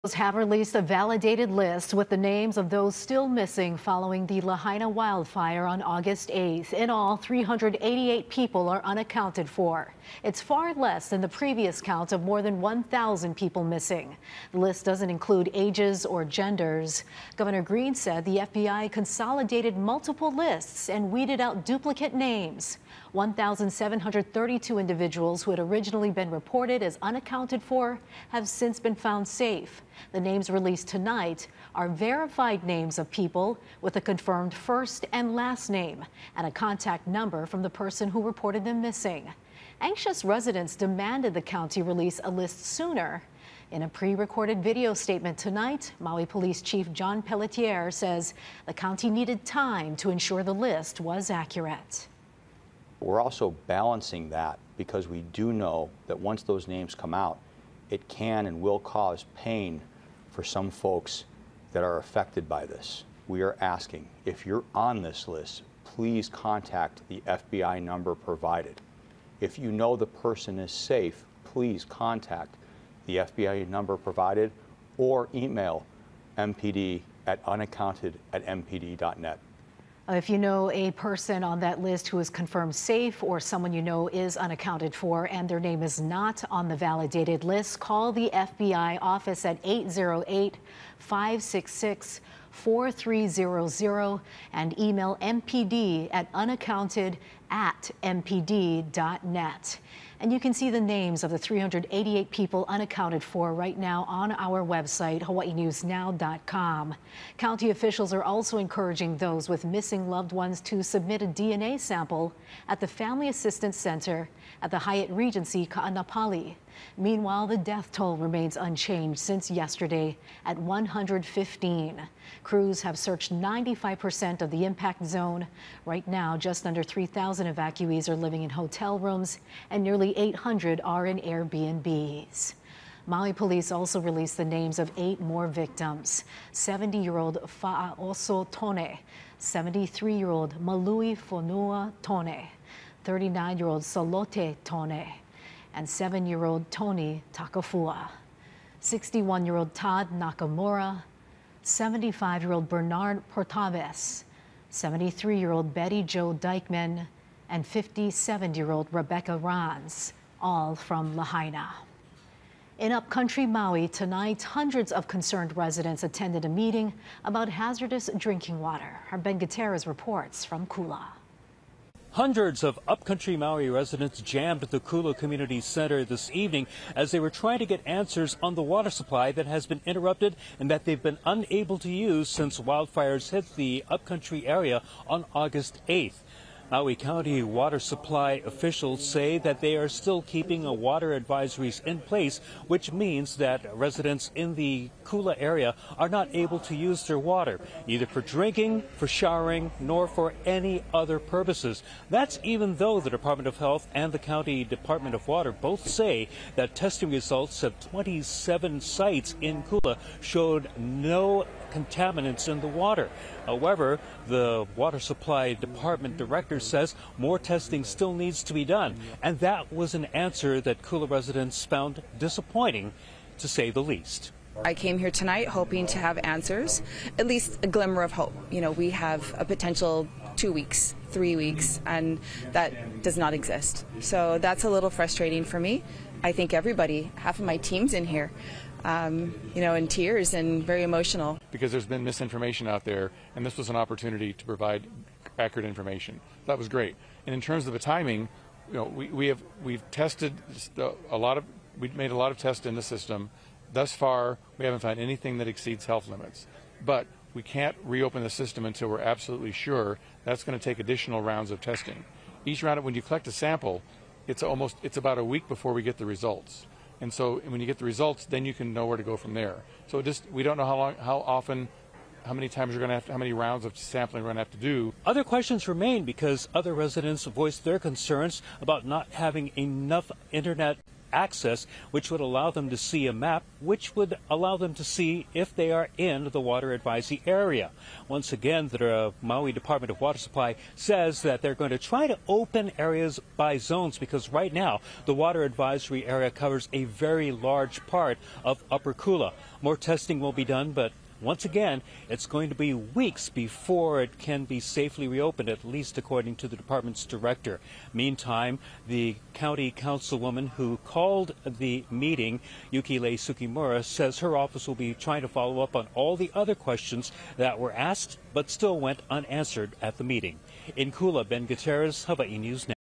Maui releases list of hundreds reported missing after Lahaina fire
Maui County has released a list of 388 people who remain unaccounted for following the ferocious wildfire that decimated Lahaina.